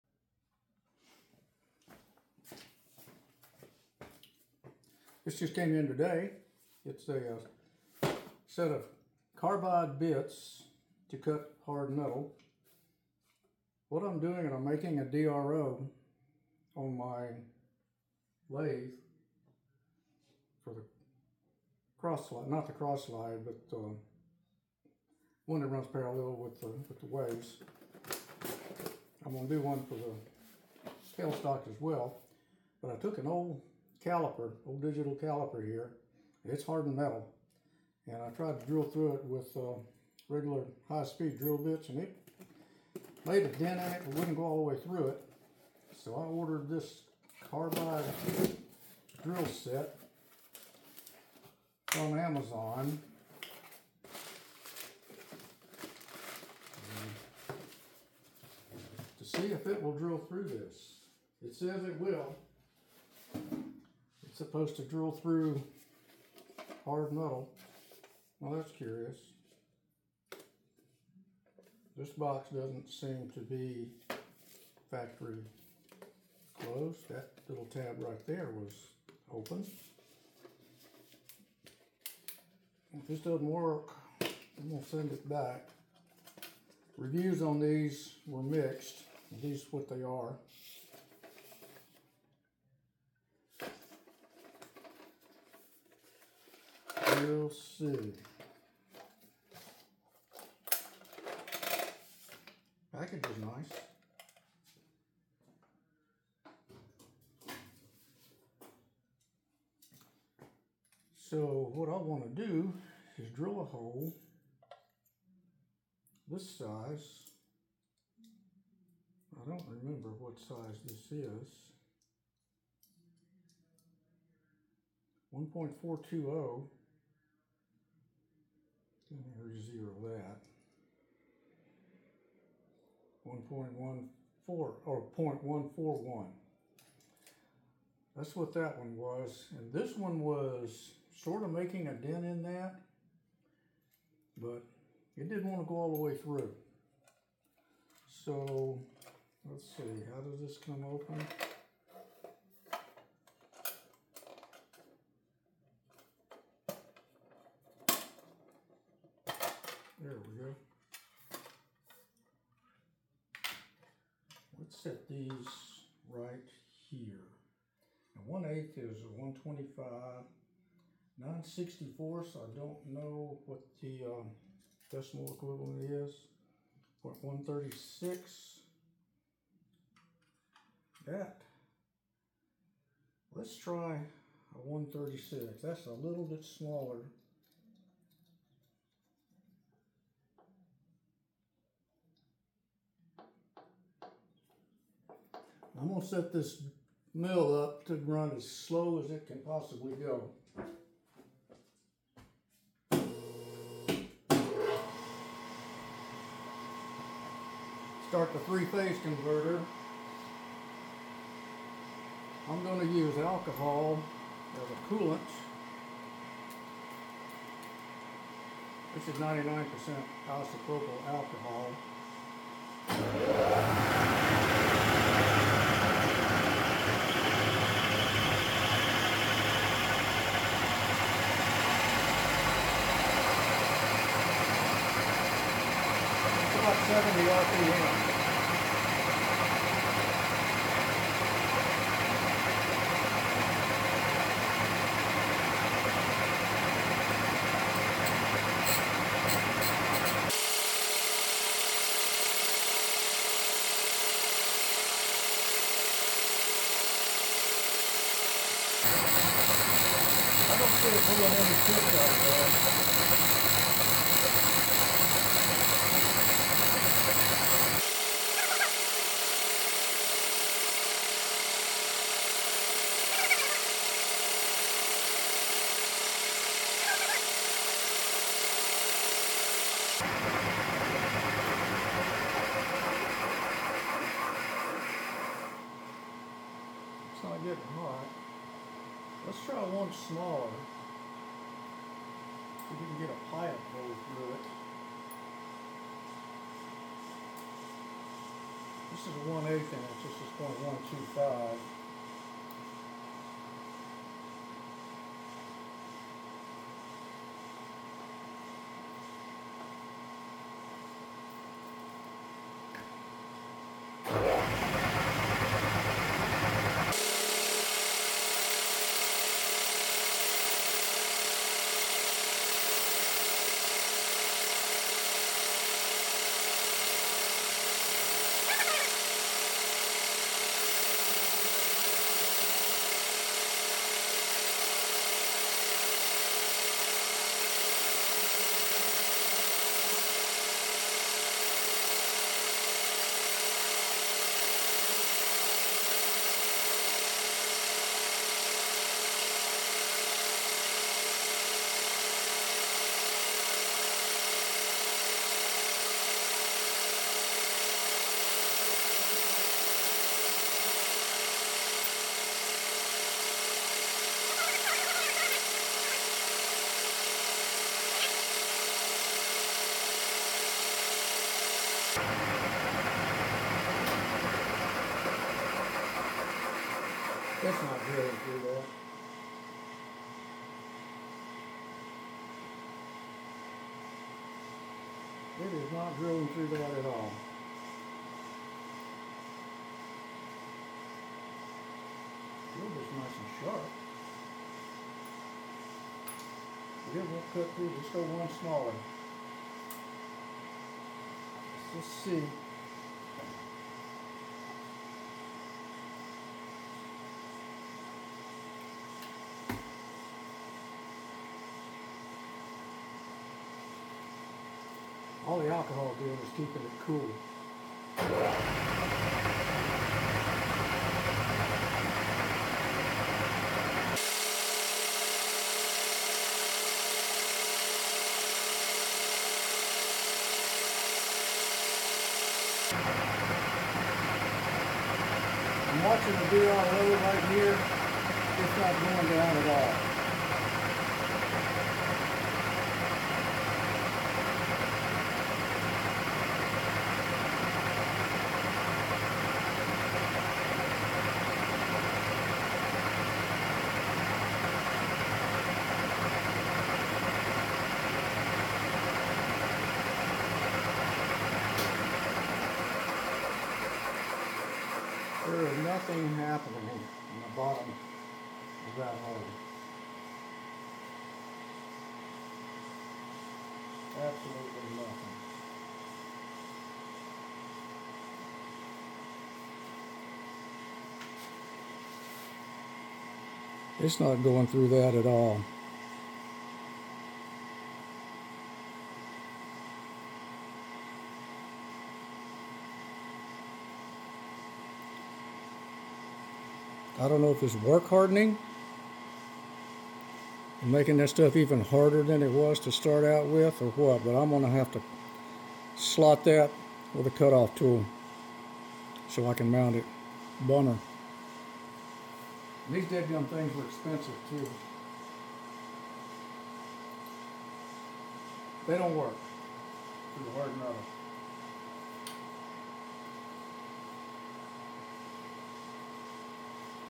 Yougfin Carbide Drill Bit Test in Hard Steel
Drilling a small (0.140" dia) hole through a digital caliper, to make a DRO for one motion on  my lathe, I ordered this set of drill bits.  It said "carbide", but on the box it says HSS (high speed steel).  Tried to finish a started hole in the steel, that is 1/8" thick, and it wouldn't do it.  Bummer.